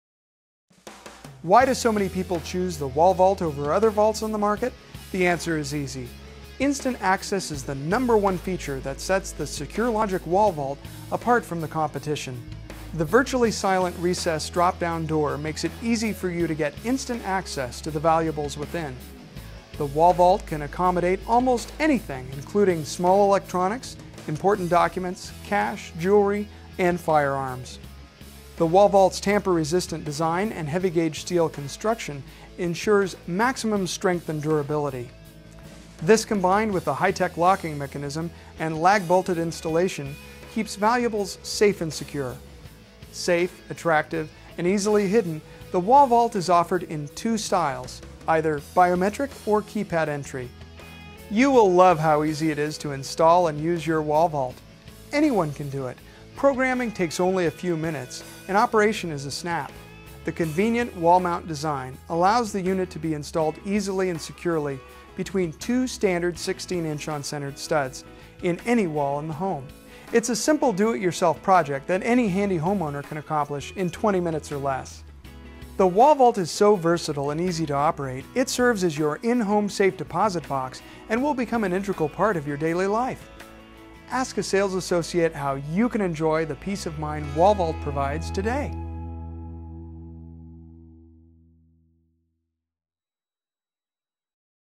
BioMetric Wall Vault
Ultra sleek, super chic and solid as a rock. The Wall Vault line of personal storage devices are designed to securely store and allow immediate access to your most commonly used valuables. The Wall Vault can easily be covered by a print or painted canvas and disguised from sight, although it's modern stainless steel appearance is quite pleasing and doesn't have to be concealed. Available with biometric fingerprint recognition or electronic keypad, the Wall Vault is YOUR secure storage solution.

Tap, touch and you're in. That's as easy as it gets in the secure storage market. Once your fingerprint is programmed into the system, which takes less than a minute, you simply touch once to activate, and touch again to read. The fingerprint recognition chip inside the system triggers an accepted read and unlocks the unit, sending the uniquely designed gravity operated door down to the open position in less than 2 seconds. It's never been easier.